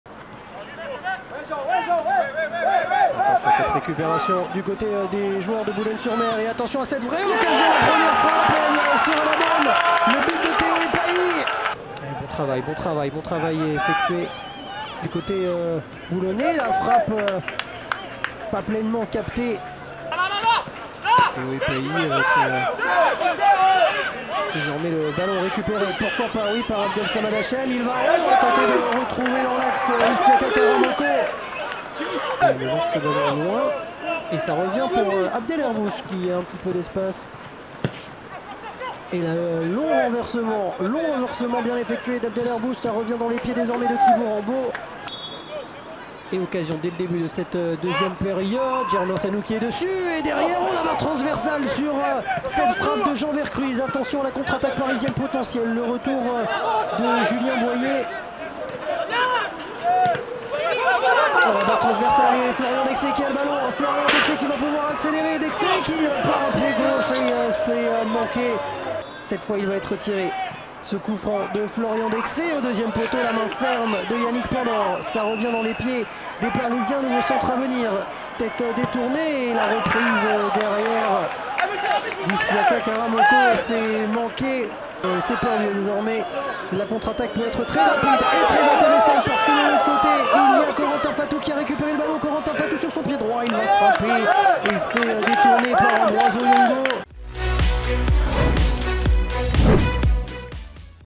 J6 I Paris 13 Atletico - US Boulogne CO (0-1) le résumé I National 2024-2025
Retrouvez le résumé de la rencontre Paris 13 Atletico Vs US Boulogne CO pour le compte de la 6e journée du championnat National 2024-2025